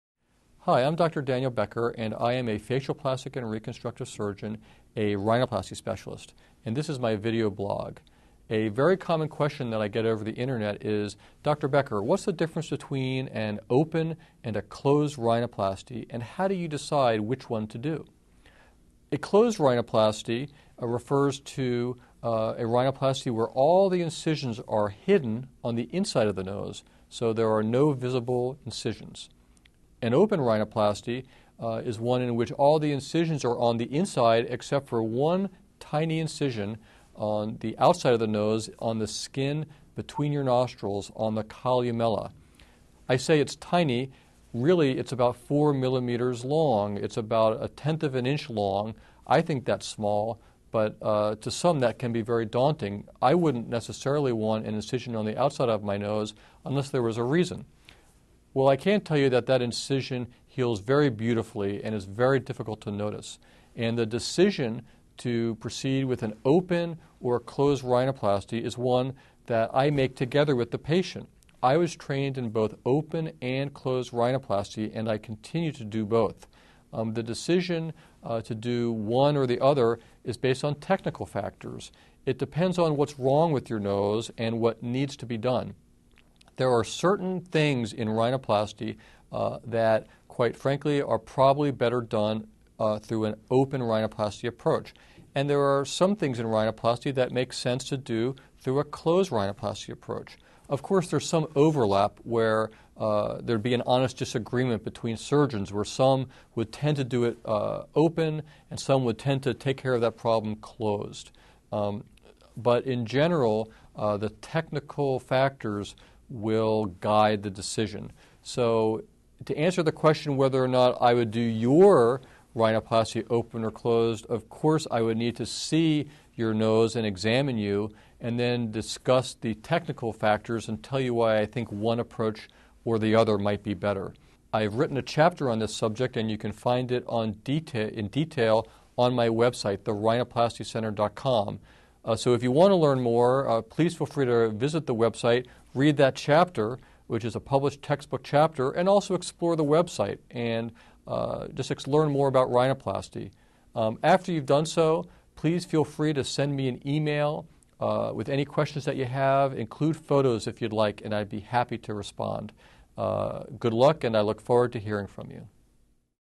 Open Versus Closed Rhinoplasty by Rhinoplasty Specialist Dr Becker in New Jersey
Dr. Daniel Becker talks about the difference between open and closed nose surgery.

or you can call any of these numbers:
856-589-NOSE (6673)